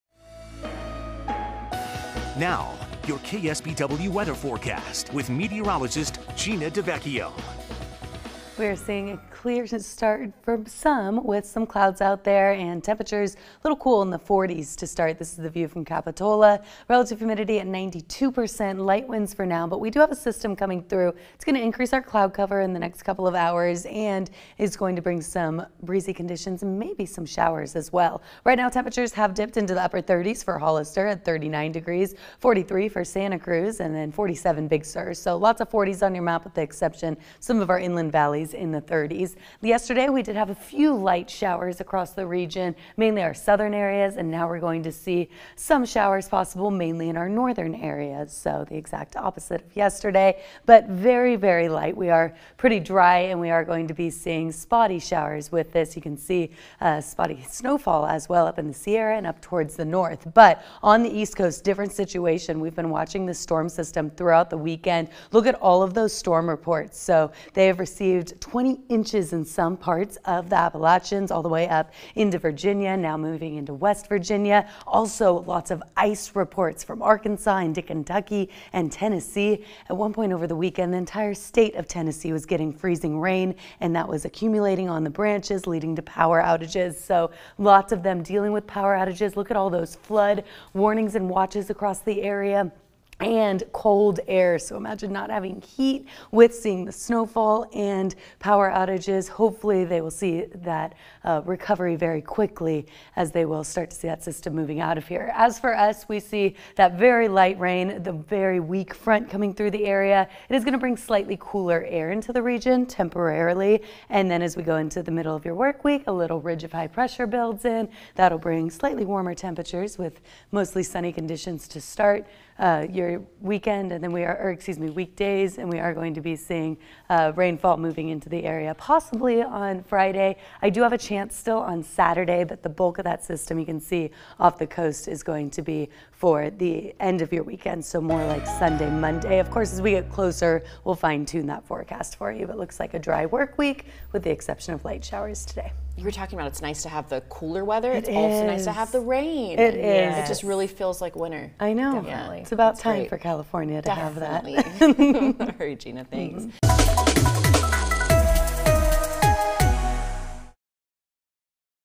Mostly cloudy, cool Monday then a sunny week ahead
KSBW Meteorologist Gina DeVecchio has your local forecast.